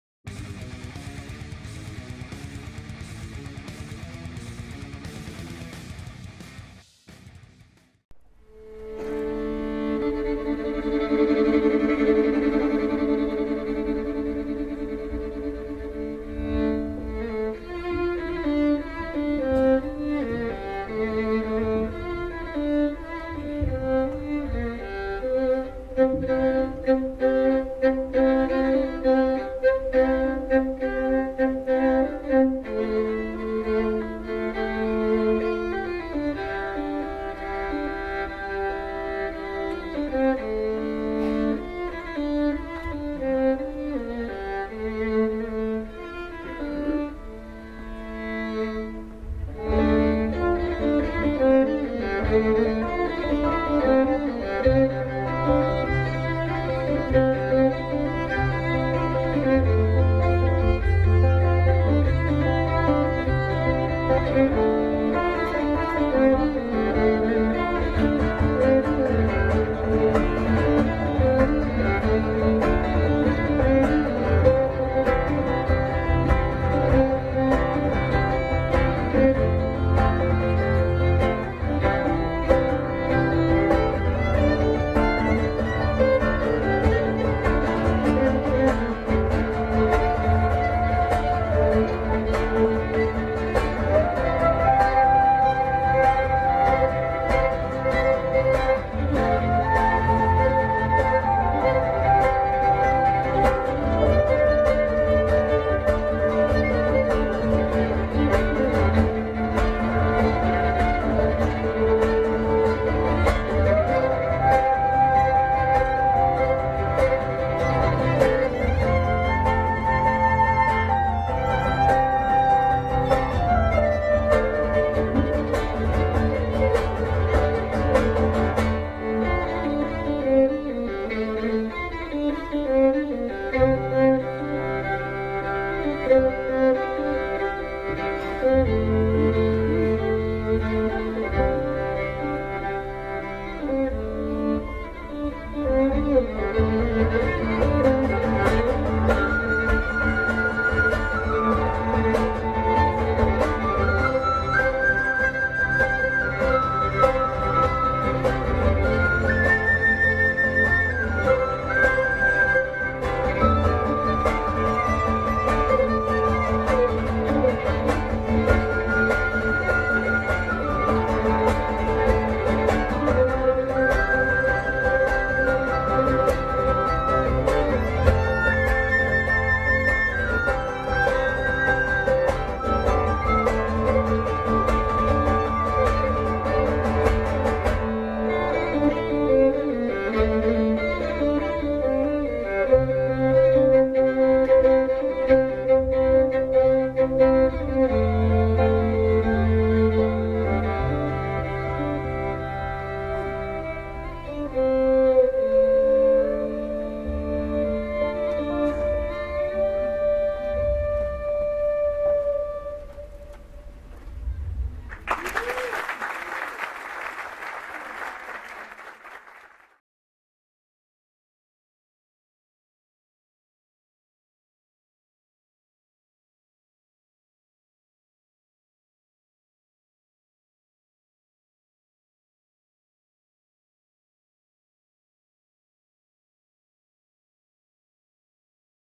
TRIO AMADEUS: Música Itinerante 2019 (Instrumental)
TRIO AMADEUS é música Mundial com personalidade mineira. Harpa, violão, violino. Música de qualidade e um lindo trabalho de vozes.
Clássico e contemporâneo, ora medieval, ora moderno.
Marcelle (harpa e voz), Fábio (violão e voz) e Rafael (violino) formam o trio que há 12 anos aposta na missão de romper as barreiras do tempo, ruindo referências seculares num estilo pessoal e contemporâneo.
A mistura de elementos é o que confere ao trabalho sonoridade e presença singulares.

O “Música itinerante” tem como proposta realizar shows intimistas e com uma programação diversificada de gêneros e ritmos musicais.
O projeto é realizado pela Favo Cultural, Imago Design e Cultura e Naza Music Entretenimento.